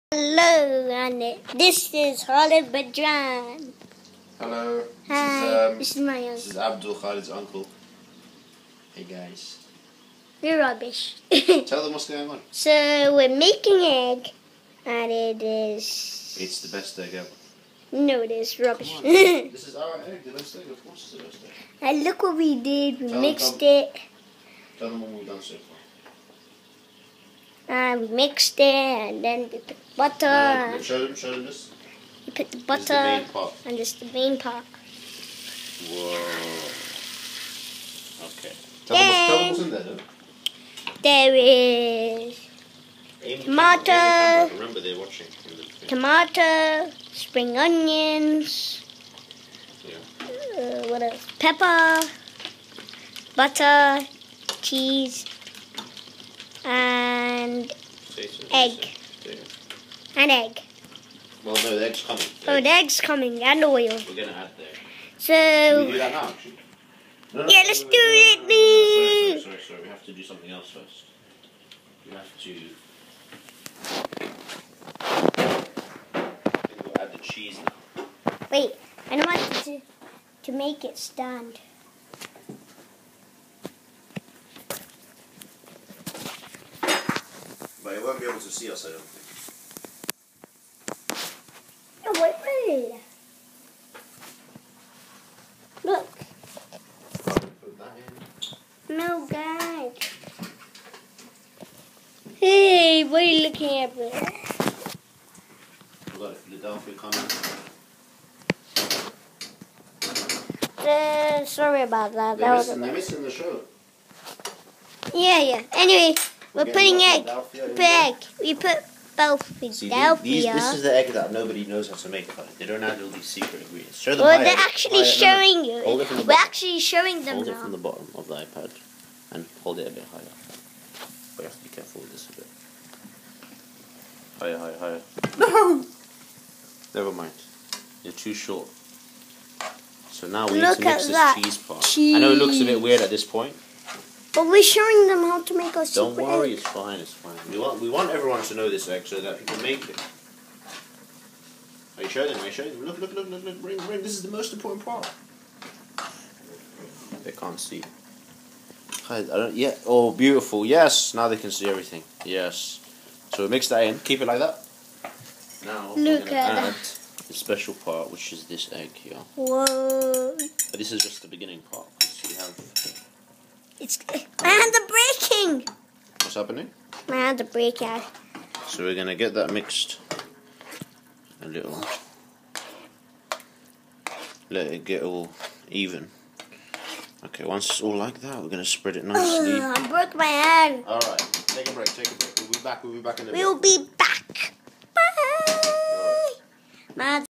Cooking with Khalid: Egg and potato omelette
Me and my uncle make an omelette! :)